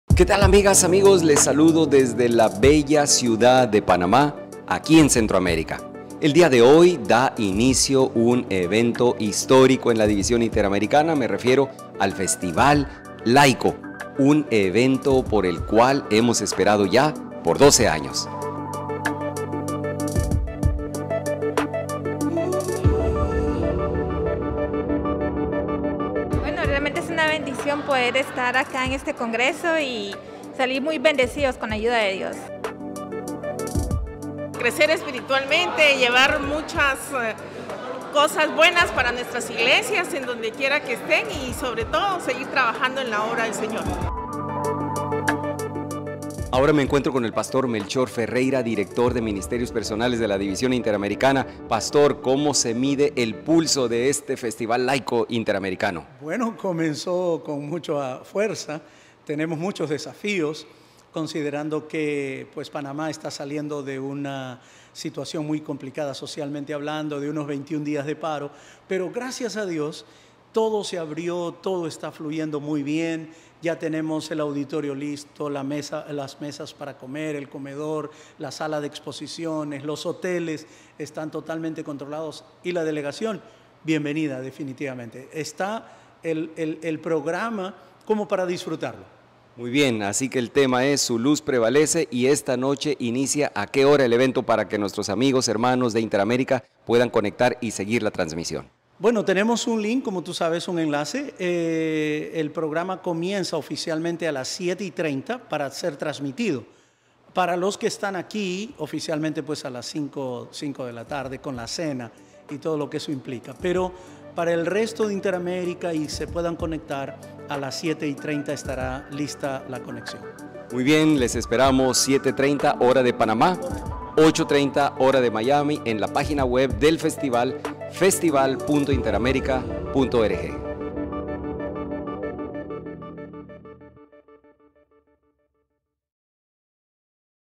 Festival Laico 2022 | Su Luz Prevalece | Día 1
Tras 12 años esperando este evento, es un gran gozo ver a todos los delegados al Festival Laico “Su Luz Prevalece” llegar a la Ciudad de Panamá a este evento que se espera sea de gran bendición no solo para ellos sino para sus iglesias locales.

Si deseas ser parte de esta bendición, sintoniza la transmisión hoy a las 8:30 PM Hora de Miami. Encuentra el link para conectarte y mucho más en festival.interamerica.org